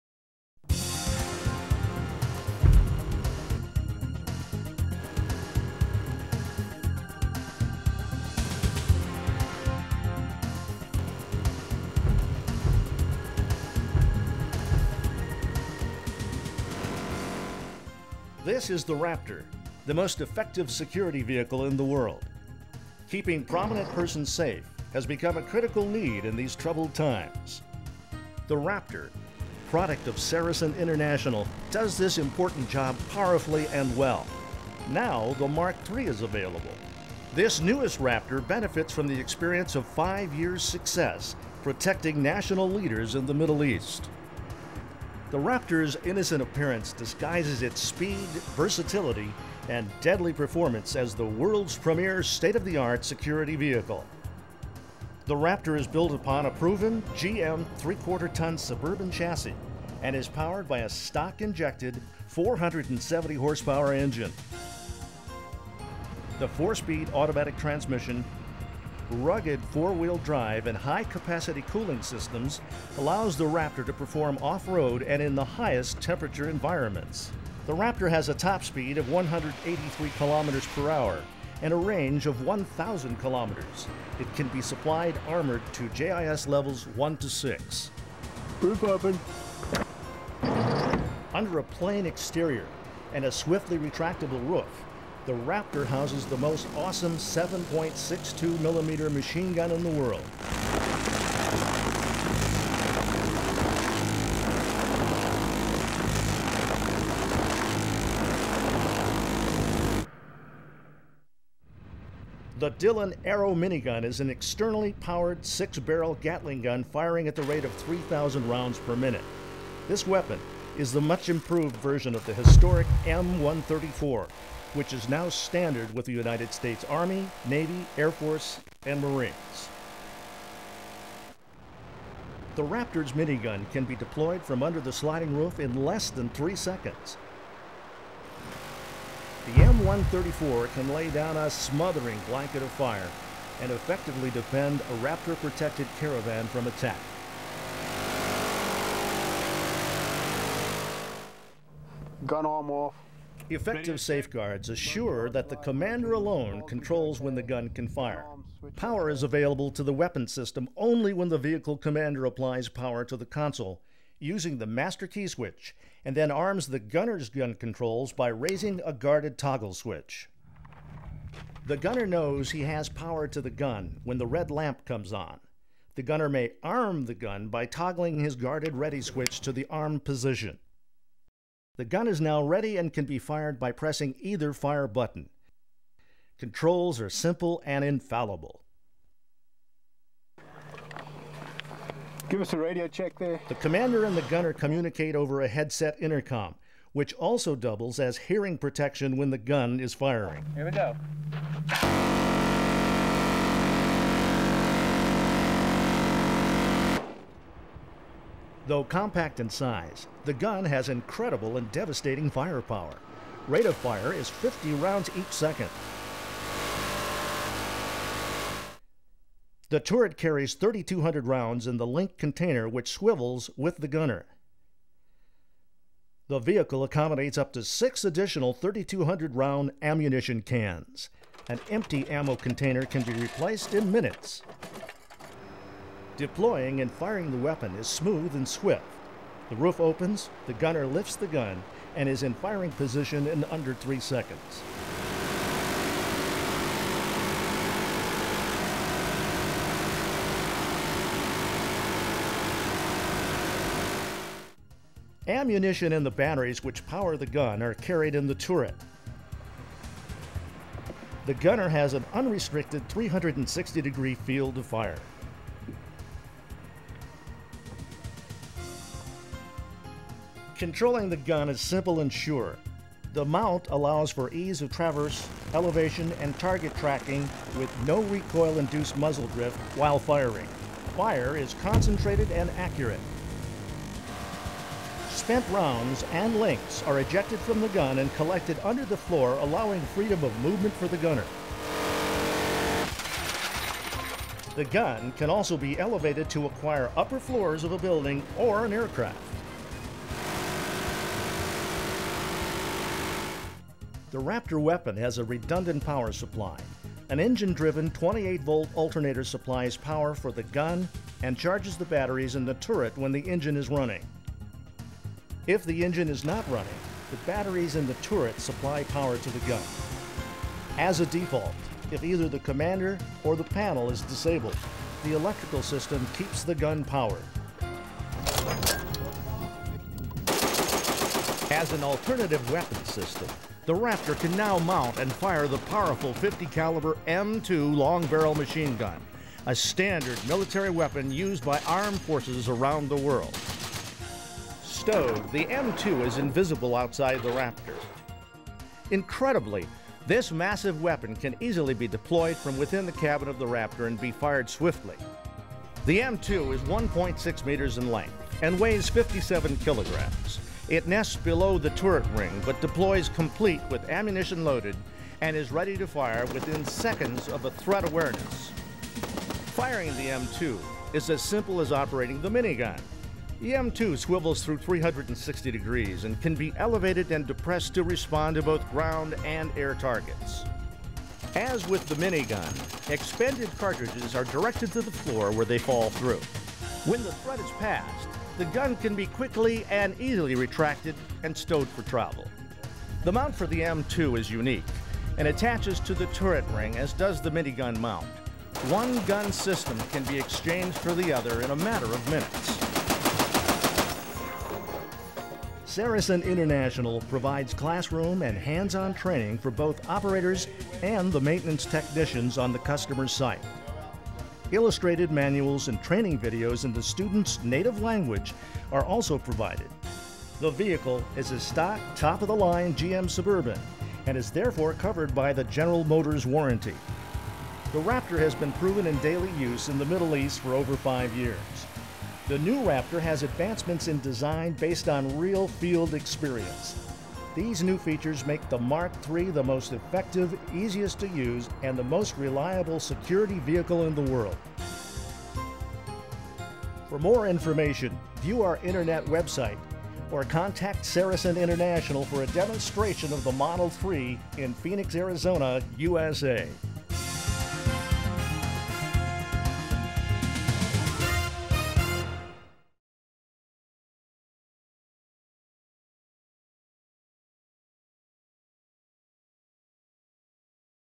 Wild Visions, Inc - 'The Raptor' video Production Phoenix, AZ
Wild Visions, Inc. of Phoenix Arizona produced this video production for Saracen International to showcase their premiere security vehicle called the Raptor. This vehicle comes equipped with an electric powered Gatling Gun capable of firing 3300 rounds per minute or a .50 caliber Browning M2 machine gun. It can effectively protect a motorized caravan or dignitaries in their travels.